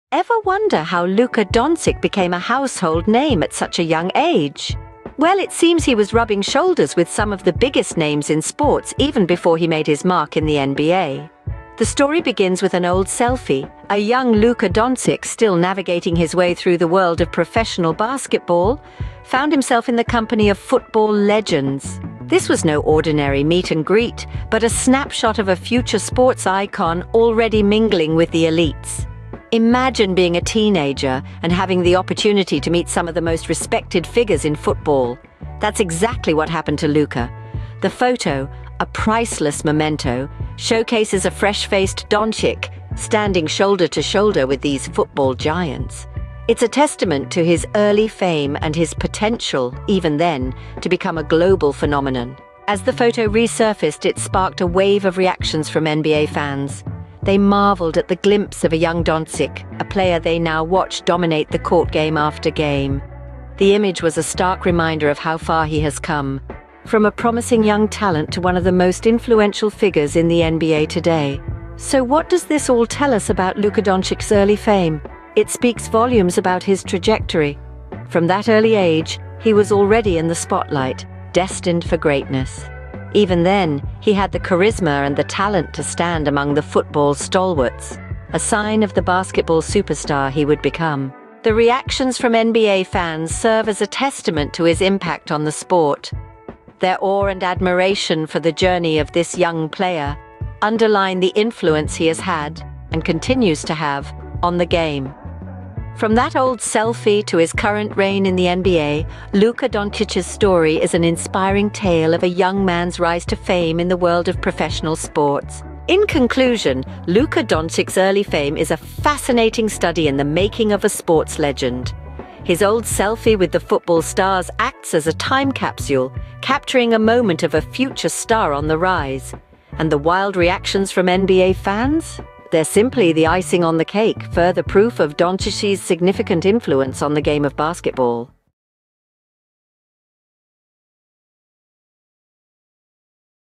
Luca Doncic's Throwback Selfie: A Fan Frenzy
In this video, we dive into the wild reactions from NBA fans after Luca Doncic posted an old selfie with famous footballers. Get ready to explore the frenzy as fans speculate and share their thoughts on this epic crossover moment!

OUTLINE:

00:00:00 The Old Selfie
00:00:52 The Resurfaced Photo
00:01:31 Impact on the Sport
00:01:55 The Making of a Sports Legend